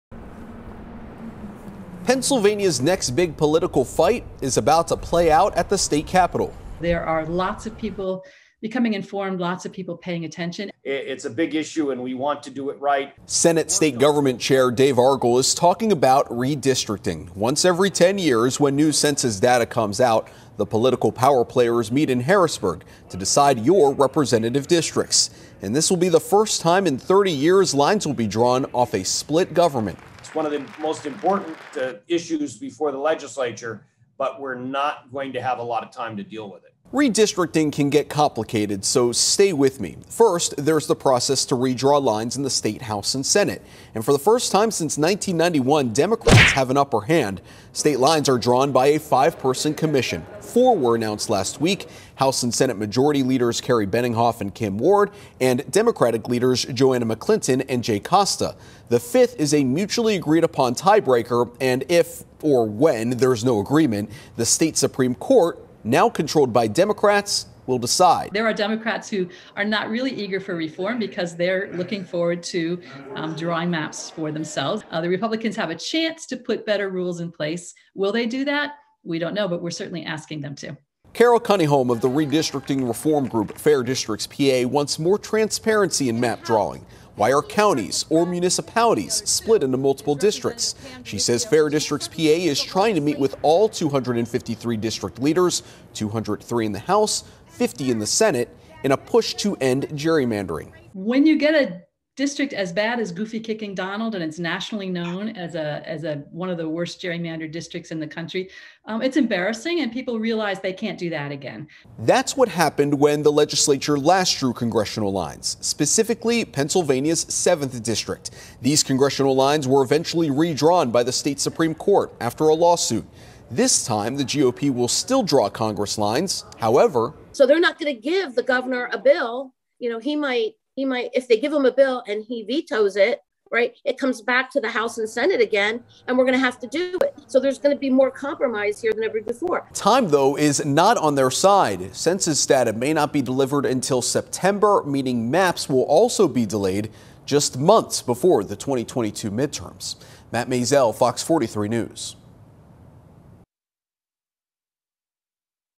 Redistricting battle looms in Harrisburg as legislative committee takes shape | FOX43 Capitol Beat
State House and Senate lines will be redrawn this year, along with Congressional lines, though experts do not anticipate much change from current set up.